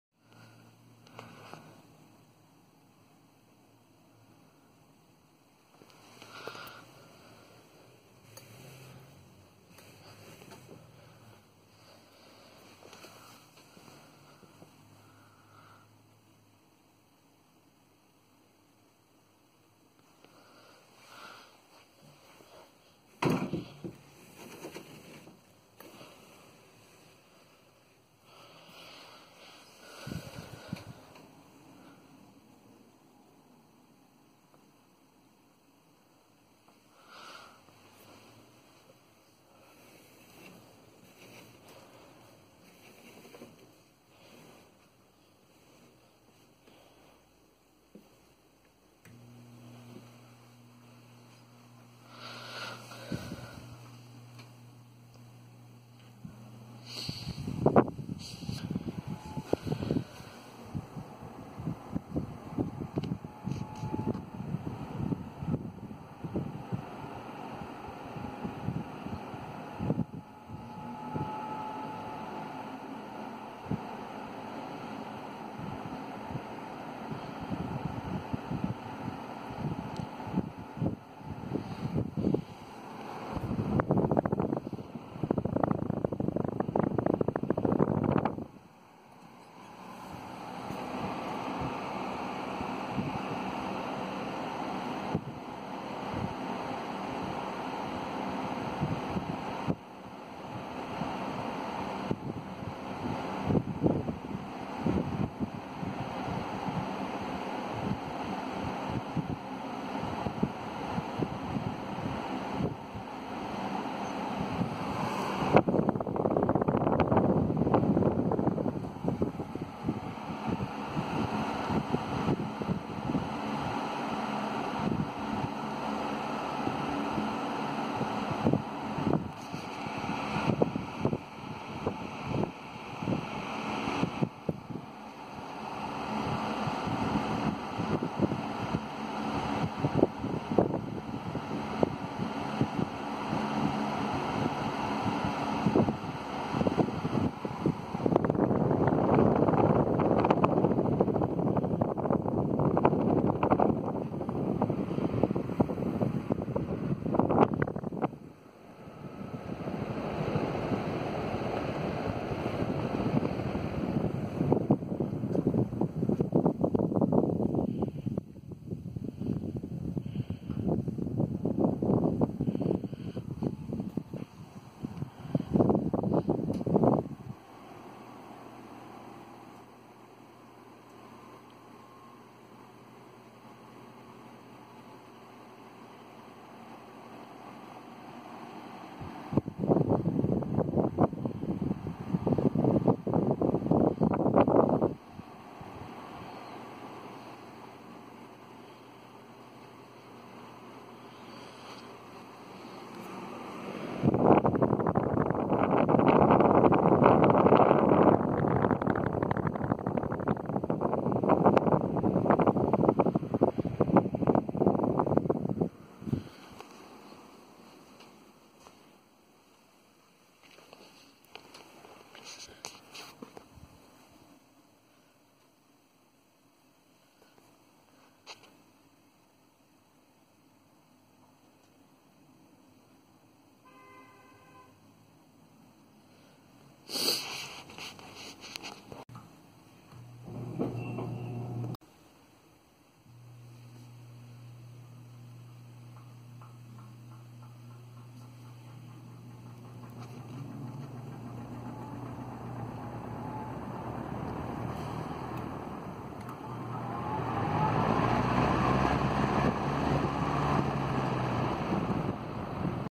Emerson Electric 16" table fan
This is the fan I restored for my mom for Christmas. The blade is a little out of balance but it is not that noticable.